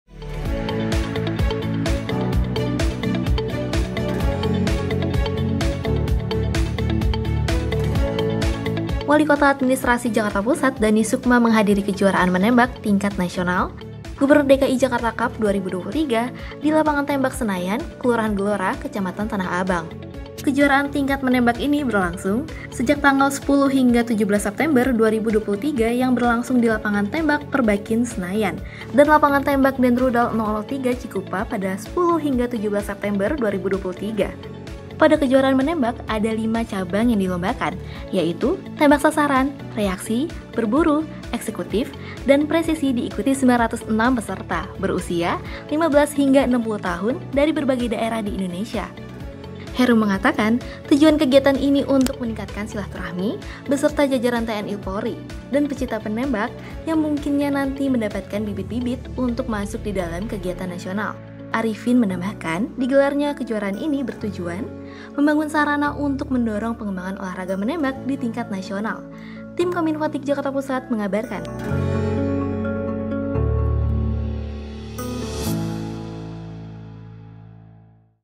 Wali Kota Hadiri Kejuaraan Menembak Tingkat Nasional Gubernur DKI Jakarta Cup 2023
Wali Kota Administrasi Jakarta Pusat Dhany Sukma menghadiri Kejuaraan Menembak Tingkat Nasional Gubernur DKI Jakarta Cup 2023, di Lapangan Tembak Senayan, Kelurahan Gelora, Kecamatan Tanah Abang, Minggu (17/9) lalu.

Kejuaraan tingkat menembak ini berlangsung sejak tanggal 10-17 September 2023 yang berlangsung di Lapangan Tembak Perbakin Senayan dan Lapangan Tembak Denrudal 003 Cikupa pada 10-17 September 2023.

Pada kejuaraan menembak ada lima cabang yang dilombakan yaitu, tembak sasaran, reaksi, berburu, eksekutif dan presisi diikuti 906 peserta berusia 15-60 tahun dari berbagai daerah di Indonesia.

Pada hari ini merupakan rangkaian terakhir dan dihadiri oleh Pj Gubernur DKI Jakarta Heru Budi Hartono. Heru mengatakan, ini merupakan kejuaraan menembak yang pertama kali serta hasil sinergi dengan Persatuan Menembak Indonesia (Perbakin) dan semoga bisa dilaksanakan secara rutin.

Di tempat yang sama, Kepala Satuan Polisi Pamong Praja (Satpol PP) Provinsi DKI Jakarta, selaku Ketua Panitia Kejuaran Menembak Tingkat Nasional Gubernur DKI Jakarta Cup 2023 Arifin menambahkan, digelarnya kejuaraan ini bertujuan membangun sarana untuk mendorong pengembangan olahraga menembak di tingkat Nasional.

Penulis Naskah : Andreas Pamakayo
Kameramen : Rio Cornelianto
Editor Video : Rendy Vhatra
Pengisi Suara : Bunga